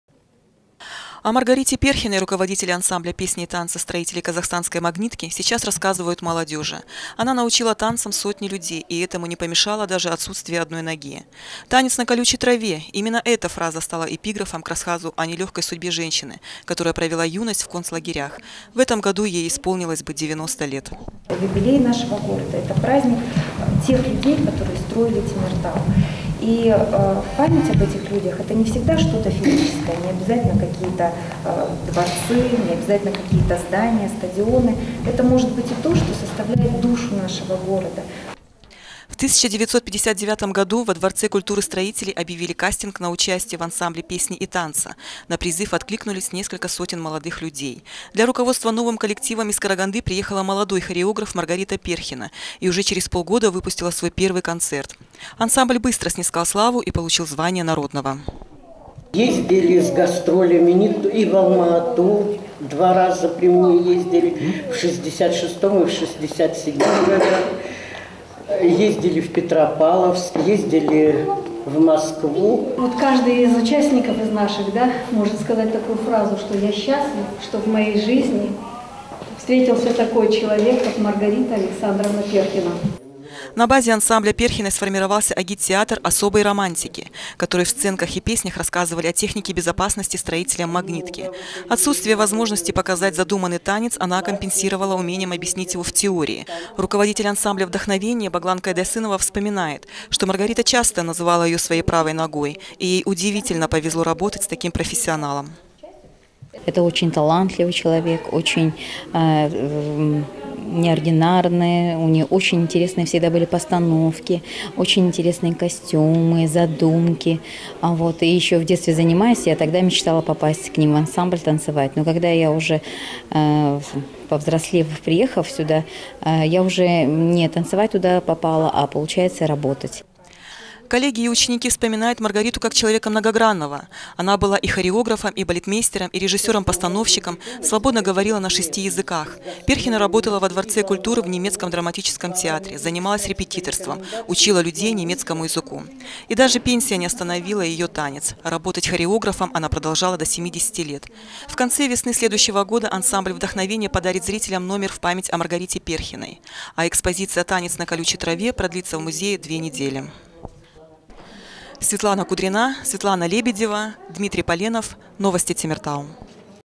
«Танец на колючей траве»
В городском историко-краеведческом музее вспоминают Маргариту Перхину. В память об известном темиртауском хореографе, который воспитал многих талантливых учеников, открылась специальная экспозиция.